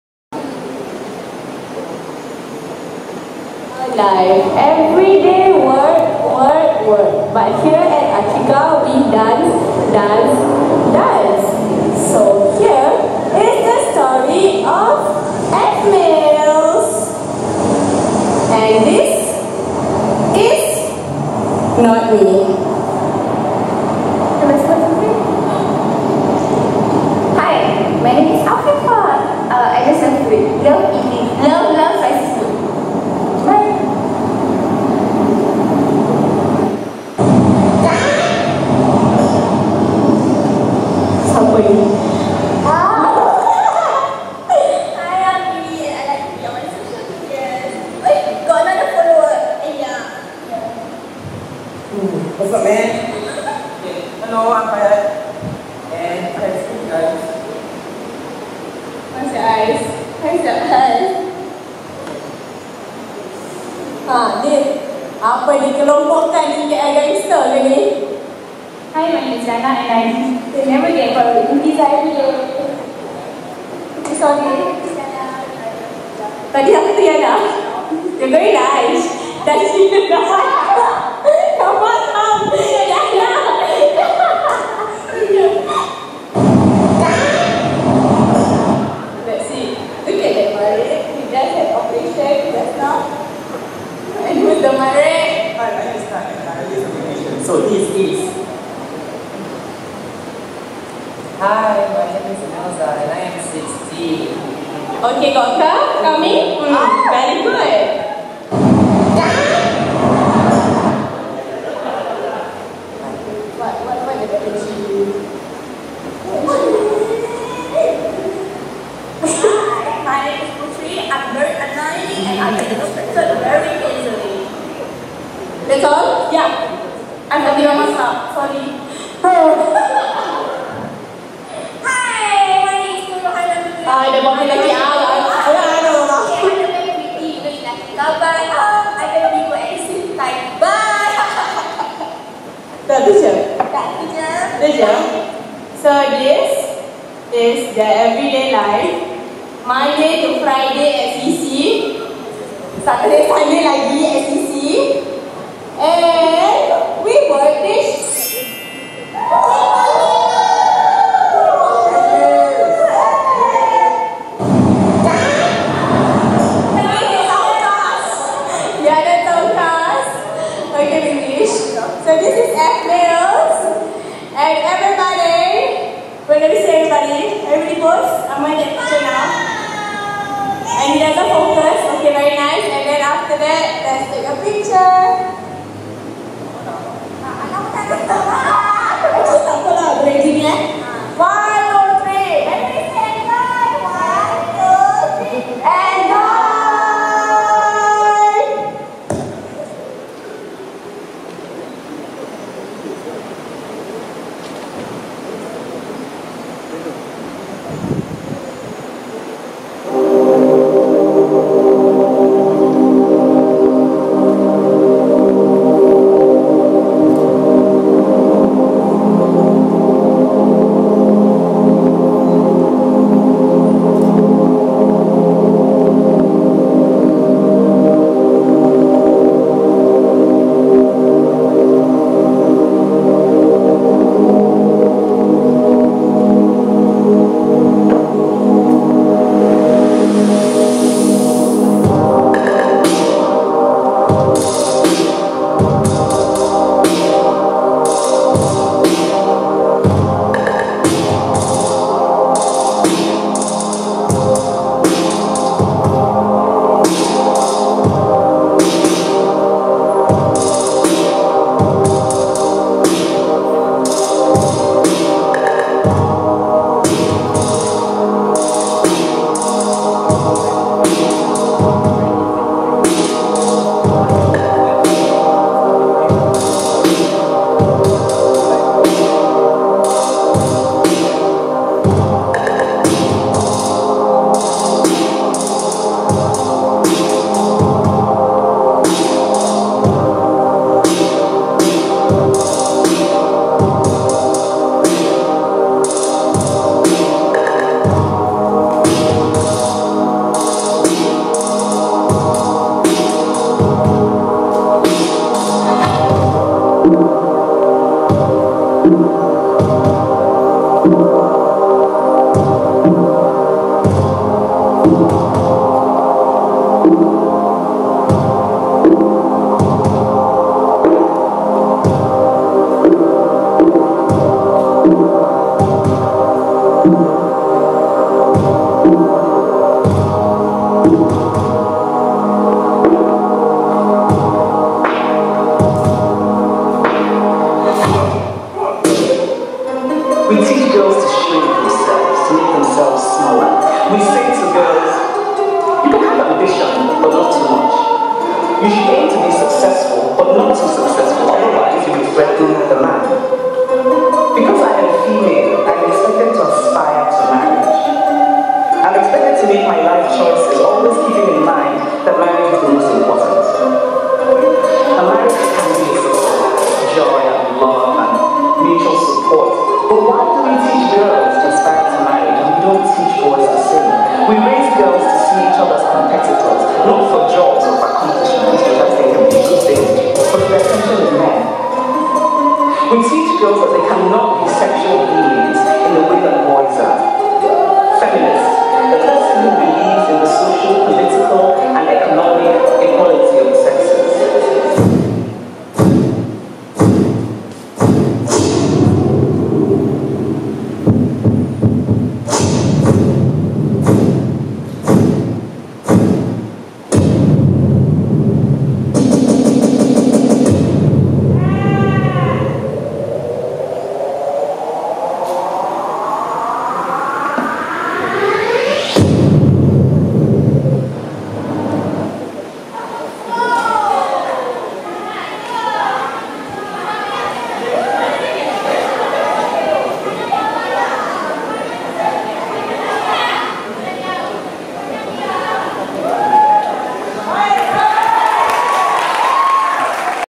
Debut Showcase 2016 - F-Males (Pamela Tan)
An introduction of the debutees, the company and the #.

Date: 19 March 2016
Venue: Kolam Ayer Community Club, Multi-Purpose Hall
Time: 9am -10pm
Main Show Time: 8pm - 9:30pm